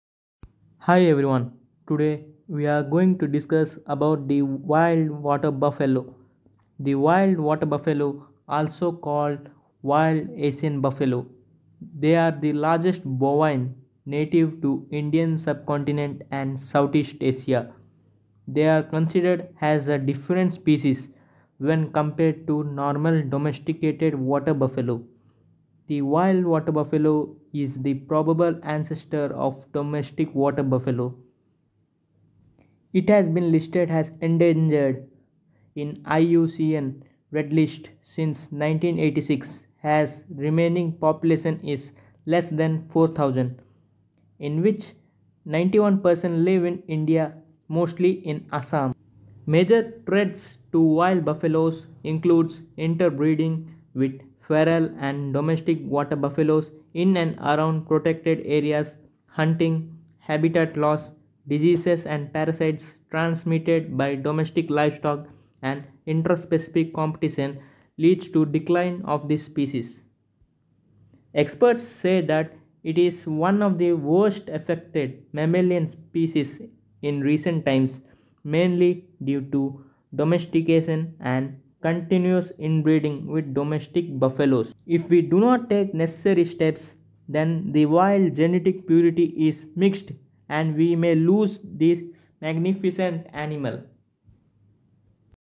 The Wild water buffalo
The wild water buffalo (Bubalus arnee) looks similar to domestic buffalo (Bubalus bubalis)but this belongs to different species . It is larger and heavier than the domestic water buffalo, and weighs from 600 to 1,200 kg .